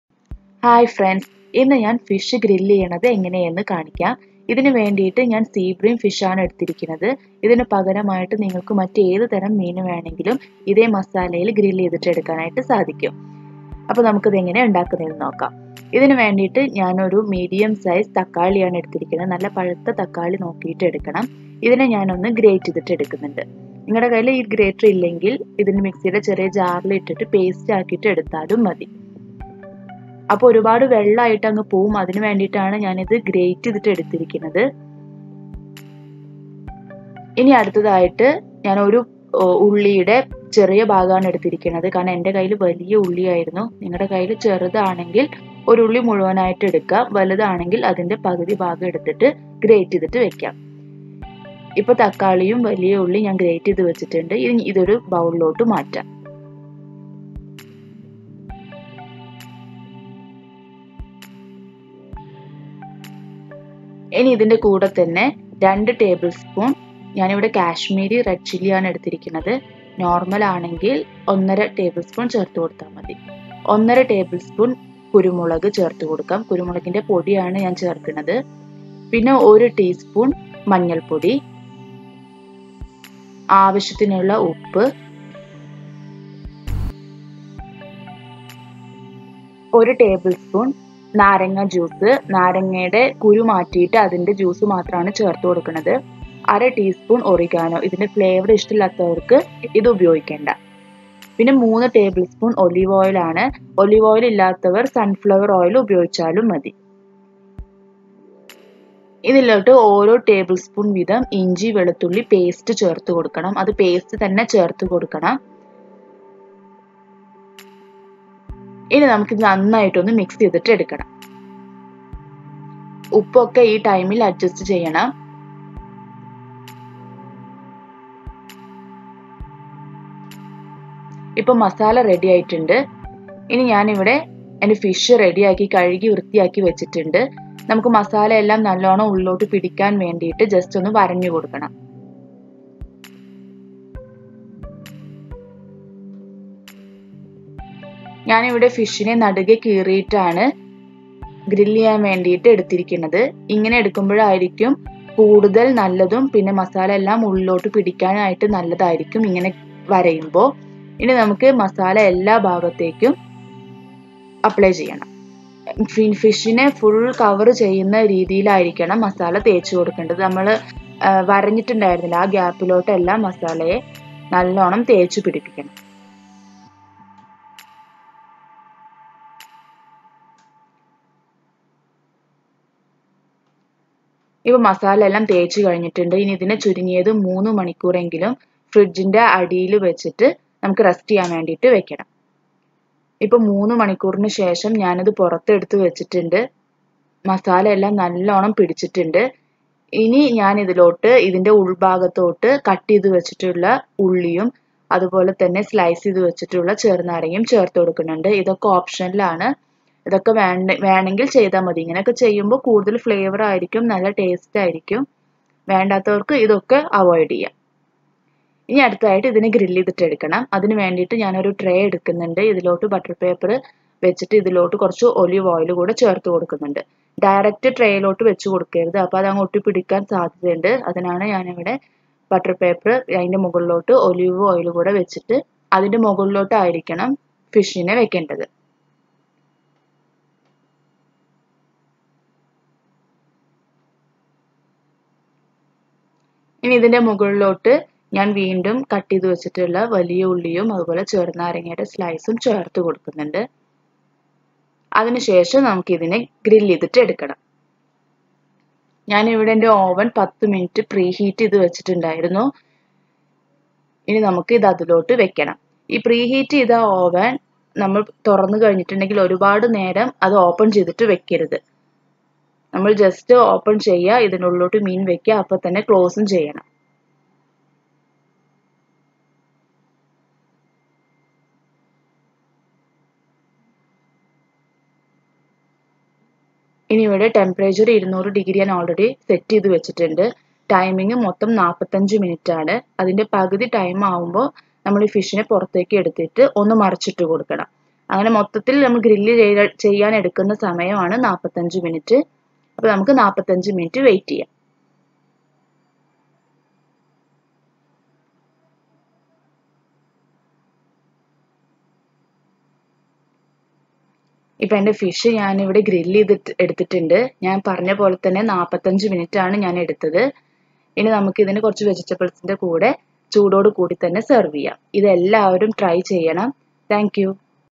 EASY AND QUICK GRILLED FISH IN OVEN
Ingredients:

Tomato-1
Onion Small Size-
salt
Oregano- 1/2 tsp (Optional)